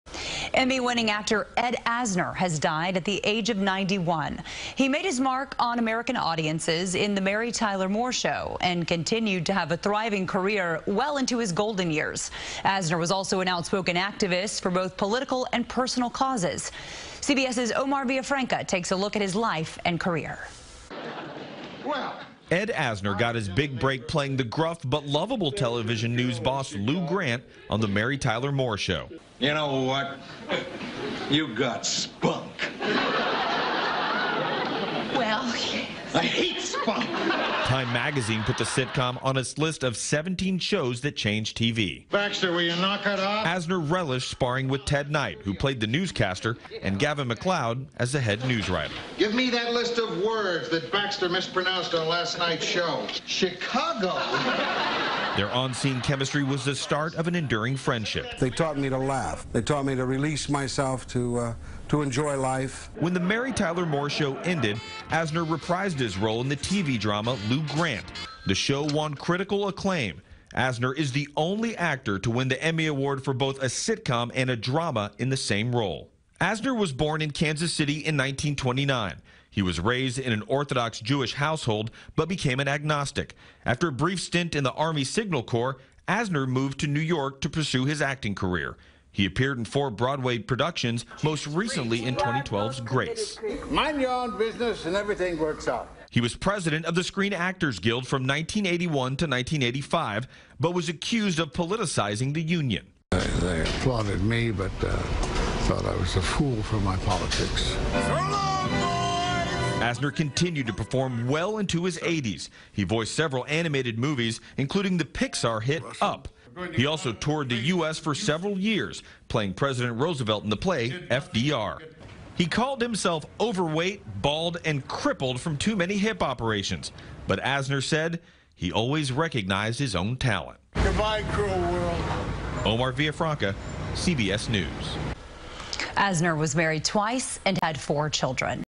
Emmy Award-Winning Actor Ed Asner Dead At 91
CBS2's Omar Villafranca has more on the TV and big screen legend's stellar career.